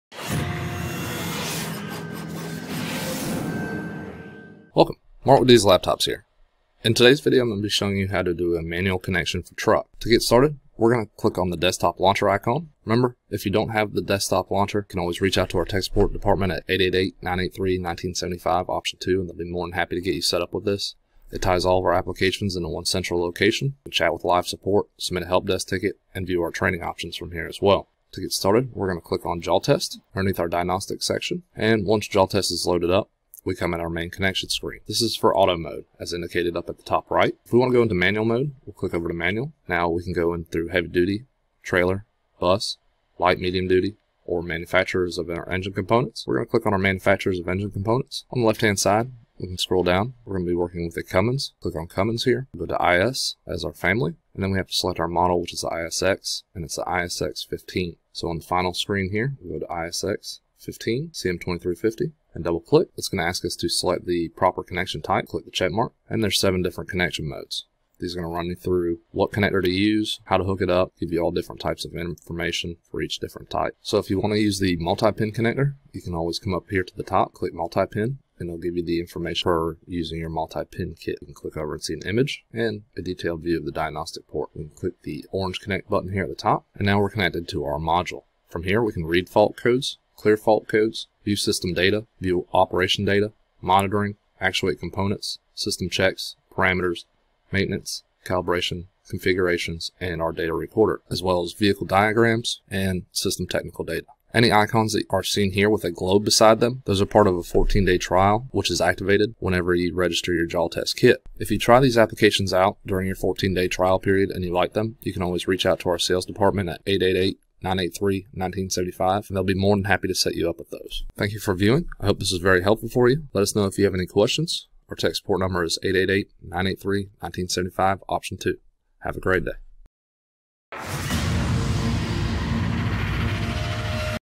Manual Connection Mode on Cojali Jaltest Truck Module
Diesel Laptops trainer Mark Lipinski demonstrates how to use the manual connection mode in the truck module of Cojali Jaltest. 2